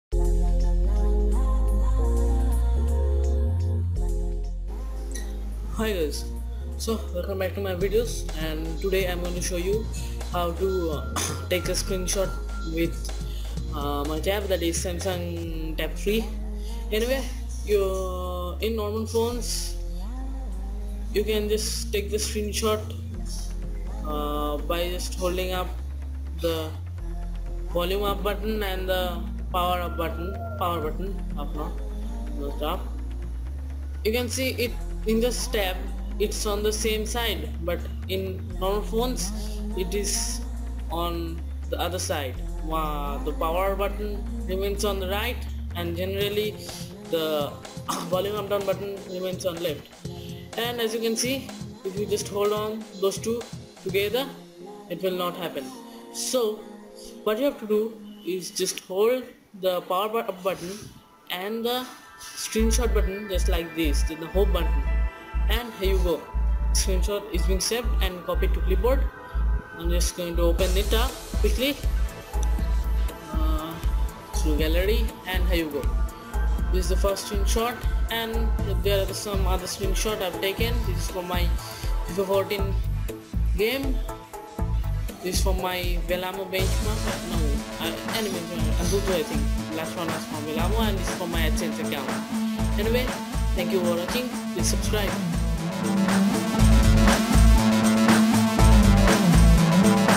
How To Take Screenshot In Tab3
In this video i'v  shown How To Take Screenshot In Tab3.Hope you like it!!